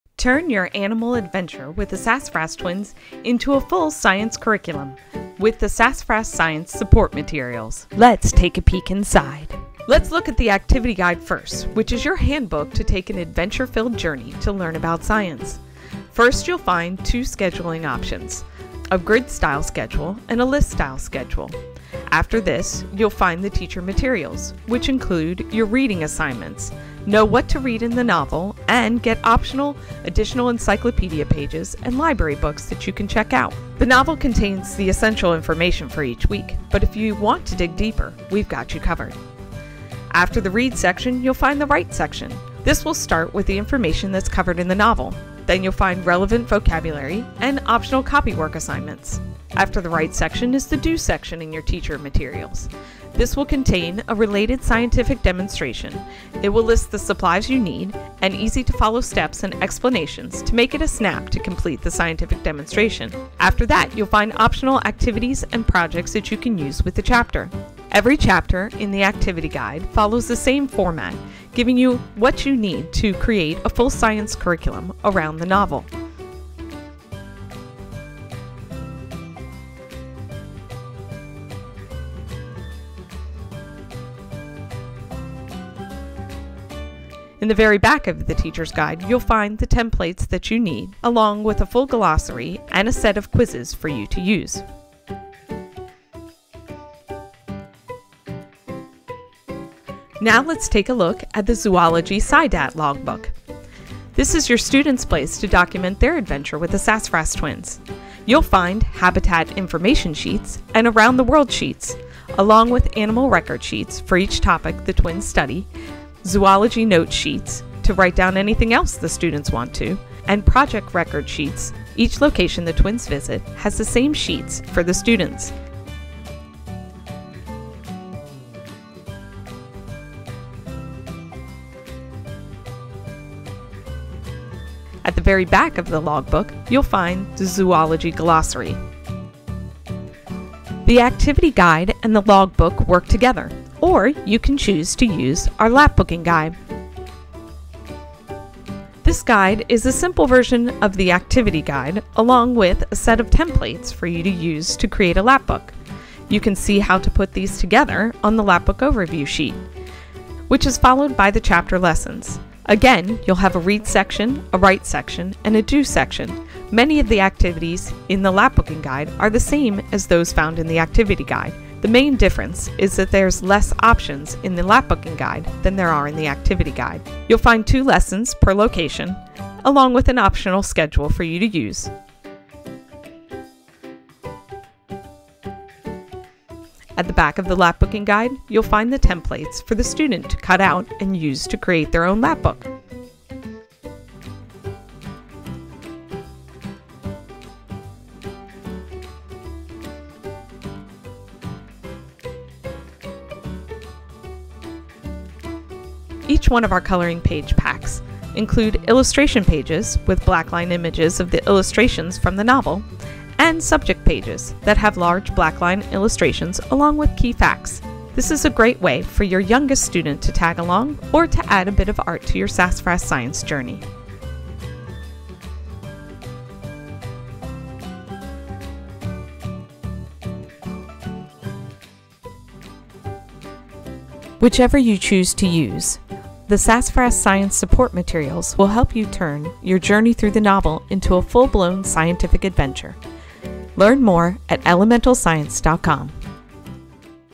See inside the Sassafras Science Zoology Materials from Elemental Science
Sassafras Science Zoology makes learning about animals easy and fun!

Get a peek inside the Sassafras Science Zoology materials. This living book science program from Elemental Science allows you and your students to take a journey to learn about science.

Head to exactly what you want to see:
00:09 - The Sassafras Guide to Zoology
01:40 - The Official Sassafras SCIDAT Logbook: Zoology Edition
02:35 - Lapbooking through Zoology with the Sassafras Twins
03:26 - Sassafras Science Zoology Coloring Pages